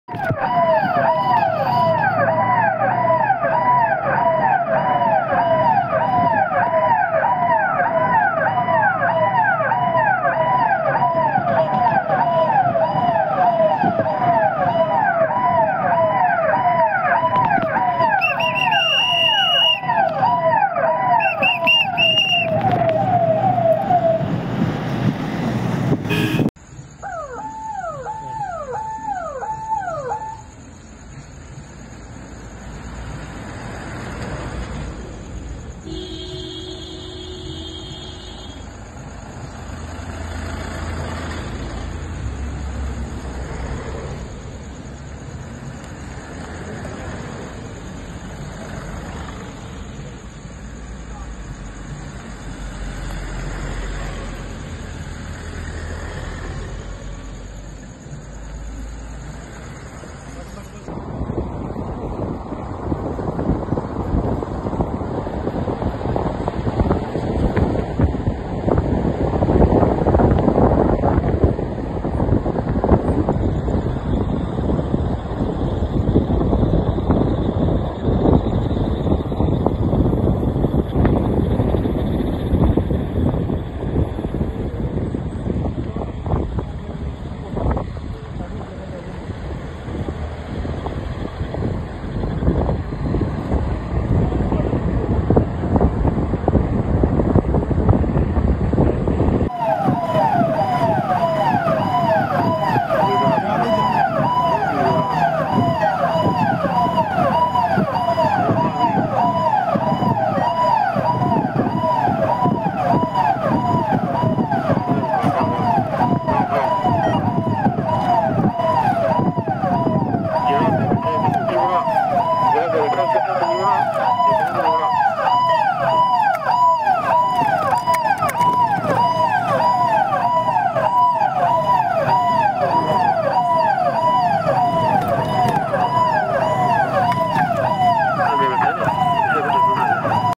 Jammu: DIG Shiv Kumar Conducts Patrolling Of Border Areas
DIG JKS  Range  Sh Shiv Kumar IPS alongwith  SSSP Samba Vinay kumar DSP HQ Dysp OPs SHO Samba Conducted intensive patrolling of Samba-Kathua Border After receiving inputs  .  He also visited different BOP s   met with BSF officials.